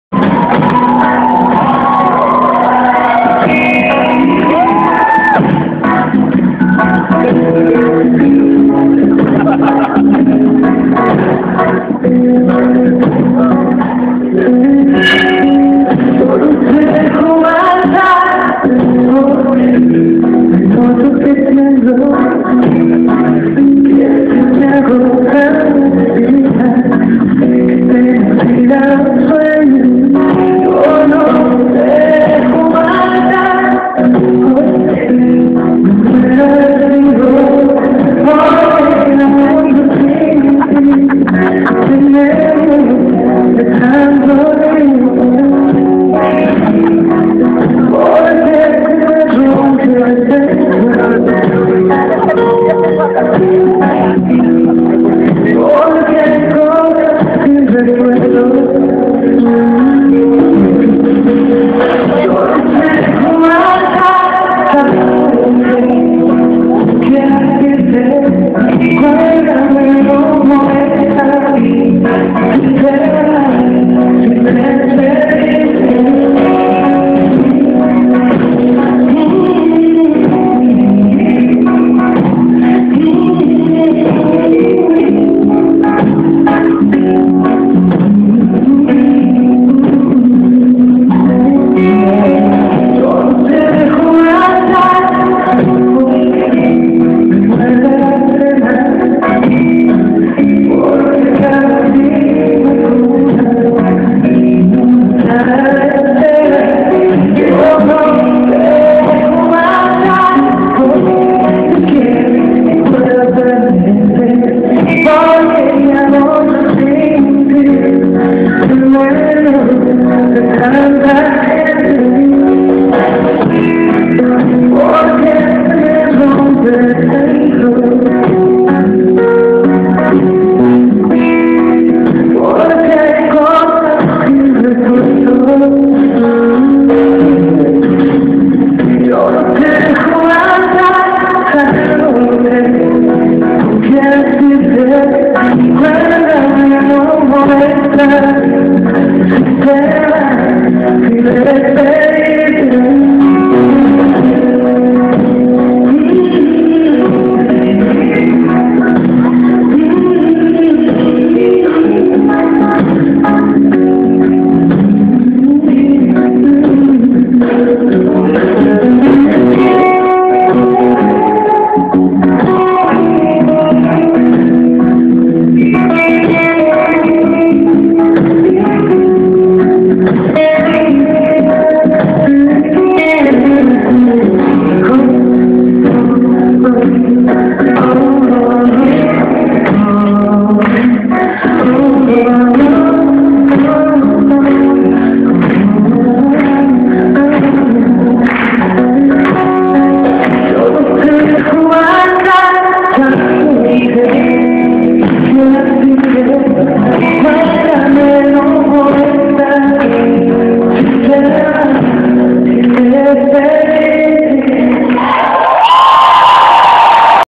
Rosana en Concierto Plaza Condesa 2
Este concierto se llevo a cabo en El Plaza Condesa el día 06 de junio de 2012